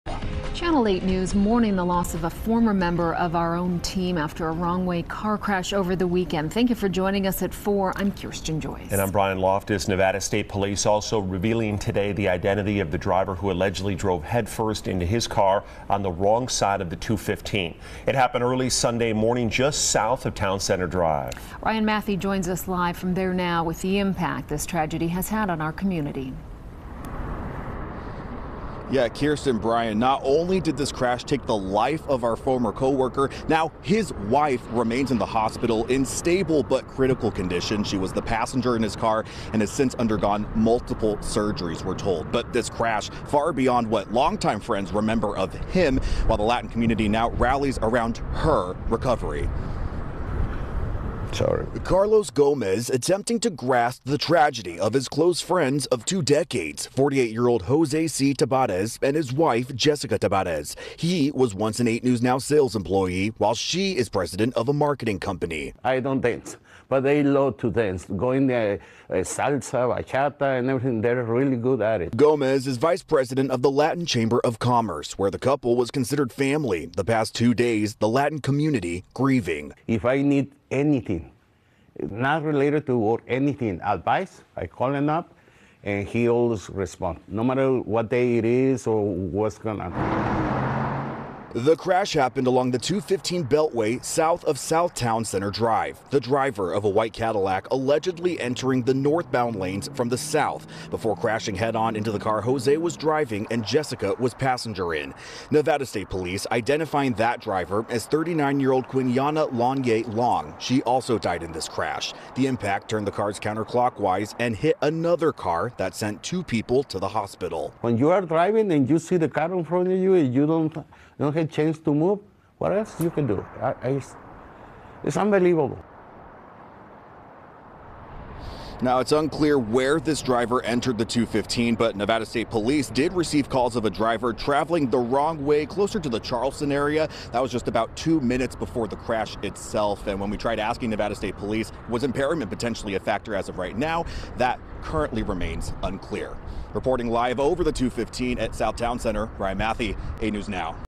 Update: 2 killed in 215 Beltway crash involving wrong-way driver identified
Both drivers who were killed in a crash on the 215 Beltway early Sunday morning have been identified. Family members identified 48-year-old Jose Cutberto Tabares-Acosta as the driver of the Chevrolet pickup. Tabares was a former 8 News Now employee.